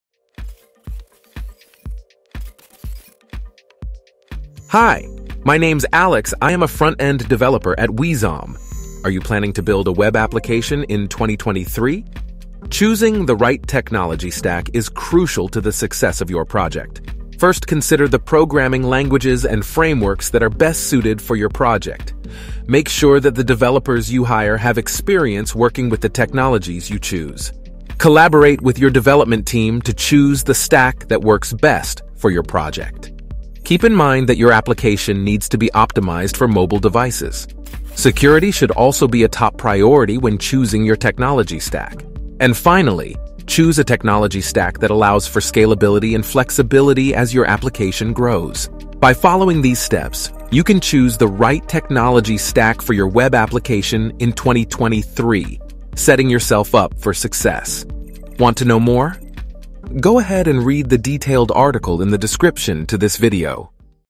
𝗛𝗼𝘄 𝘁𝗼 𝗖𝗵𝗼𝗼𝘀𝗲 𝗮 𝗧𝗲𝗰𝗵𝗻𝗼𝗹𝗼𝗴𝘆 𝗦𝘁𝗮𝗰𝗸 𝗳𝗼𝗿 𝗪𝗲𝗯 𝗔𝗽𝗽 𝗶𝗻 𝟮𝟬𝟮𝟯
At WEZOM, we know that choosing the right technology stack is crucial for building successful web applications. That's why our team has written a comprehensive guide on how to choose a technology stack for web apps in 2023

The article covers everything from front-end and back-end technologies to hosting and deployment options. Whether you're a seasoned developer or just starting, our guide will help you navigate the complex world of web app development and make informed decisions about the technology stack for your specific needs.